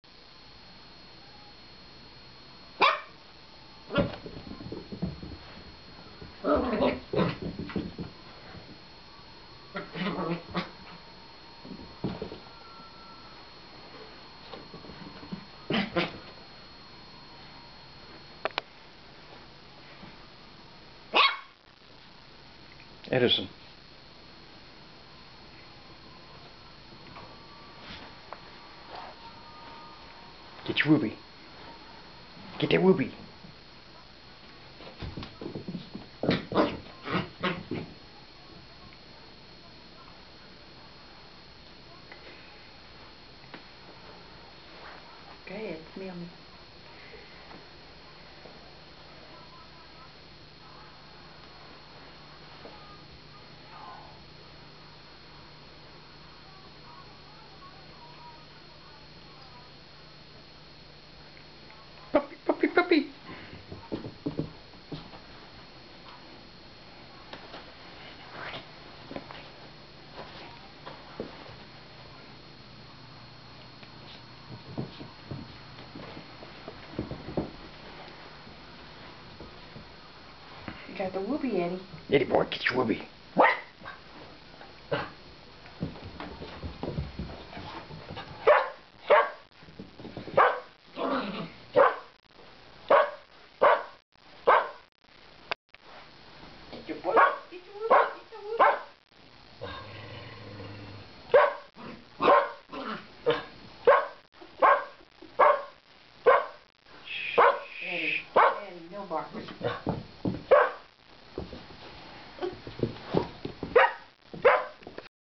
SRCI Puppy Morgan and Foster Brother Edison
Momma has gone home and Morgan has found a new friend.  Edison plays sooo rough we really have to be careful